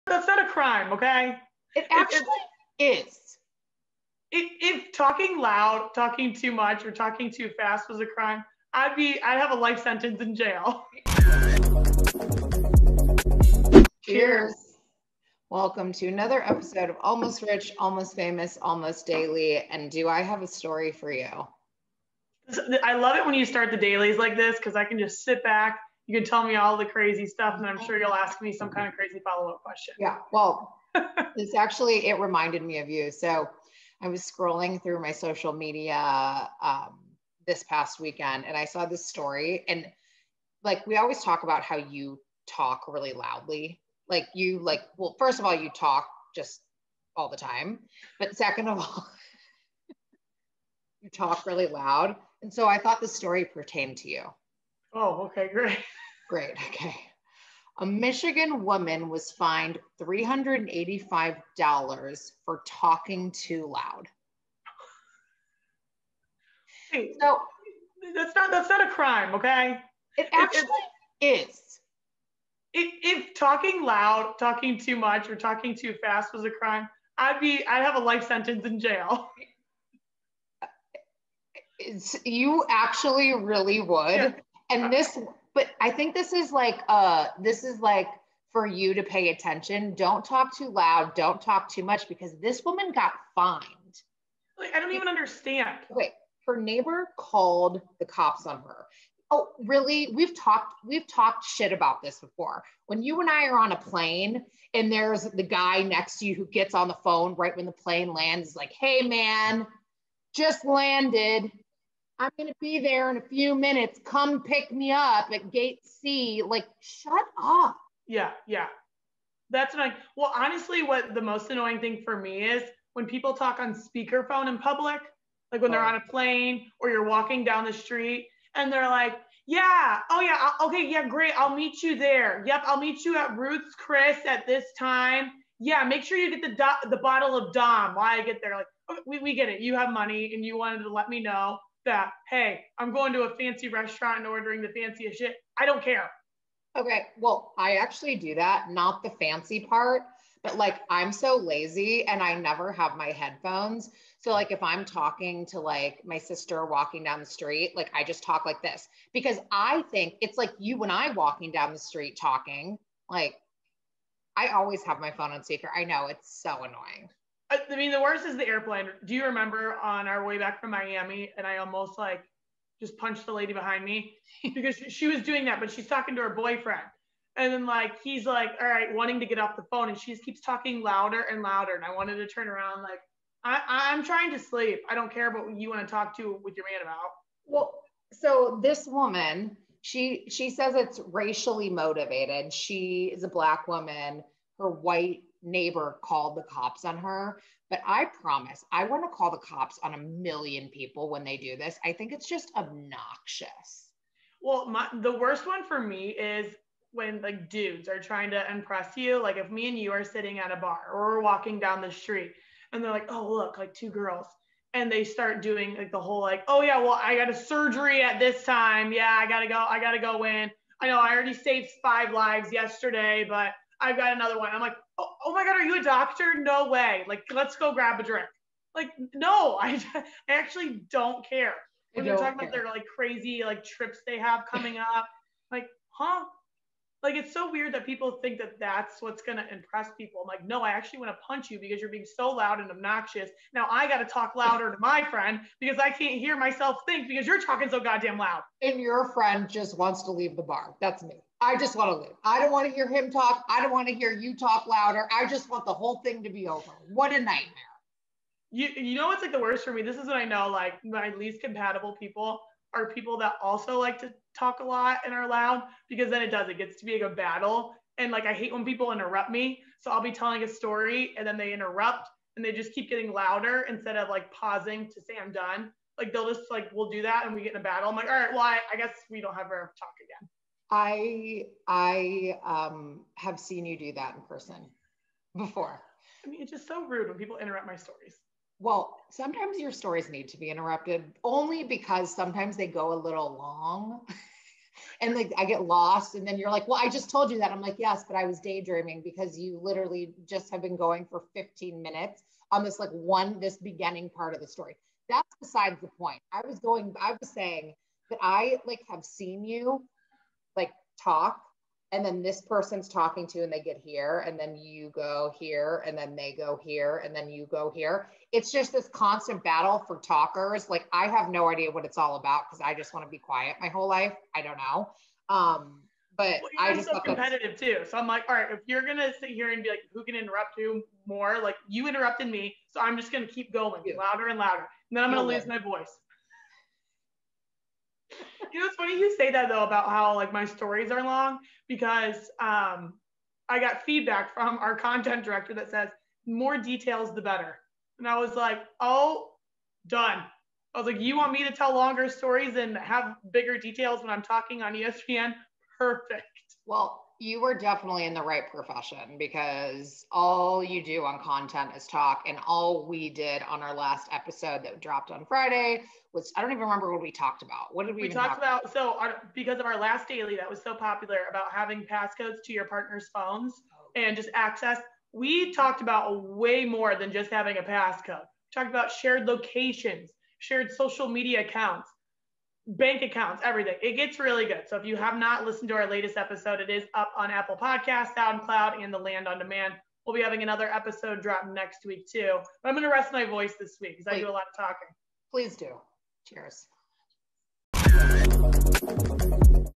Quiet down over there!
The girls discuss Michigan woman who was fined for talking too loudly, and the topic quickly morphs in to the girls pet peeves while people are on the phone. With topics ranging from speakerphone in public to ridiculously long stories, check out this fun daily video!